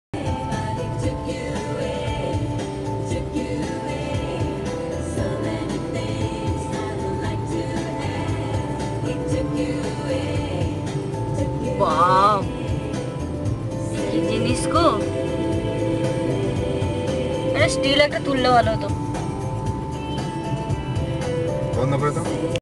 Zion National Park to Bryce Canyon - Scenic Route 9 Utah - Part2
Trip of Sumanta and Kakoli on 07-Sep-2009. Zion National Park to Bryce Canyon - Scenic Route 9 Utah - Part1